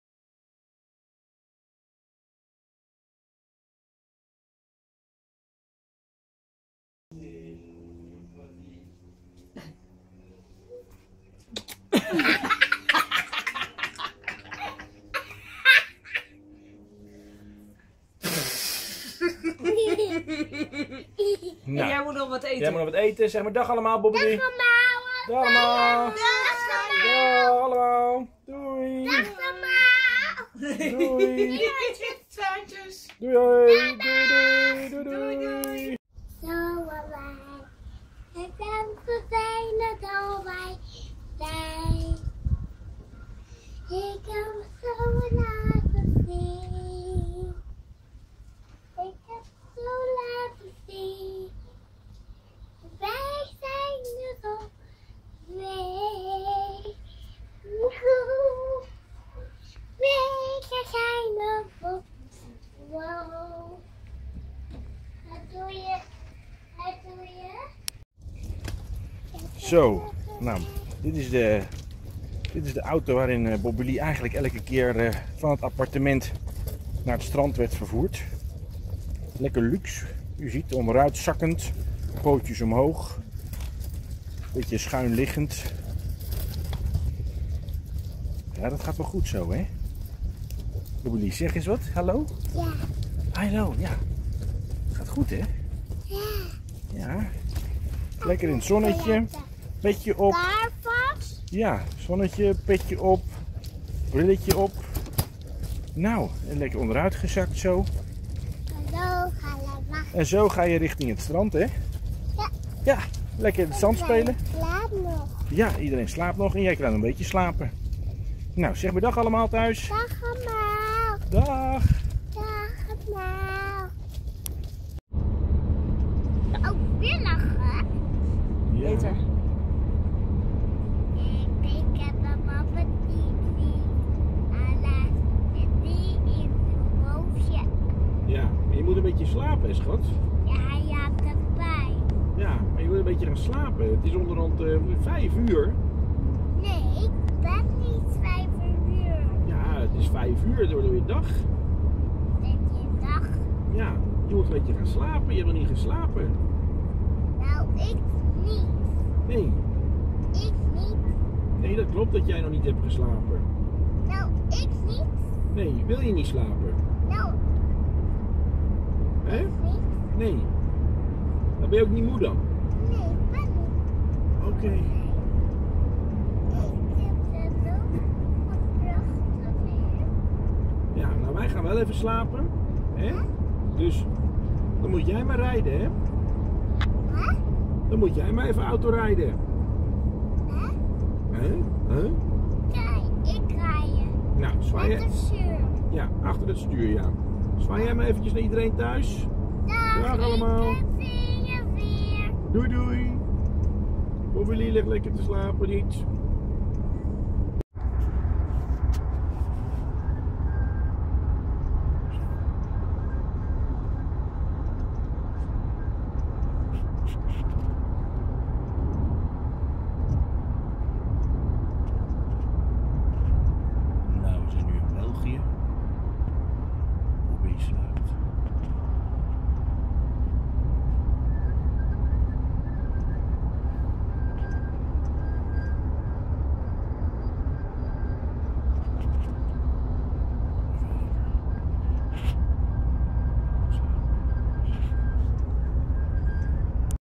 De laatste dagen van de vakantie in Zuid Frankrijk. #120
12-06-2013. En de overige dagen van de vakantie. Samen met de Mama's lekker aan ontbijt, op weg naar het strand en natuurlijk de terug weg waar Bobbi-lee in de nacht niet sliep.
12 juni 2013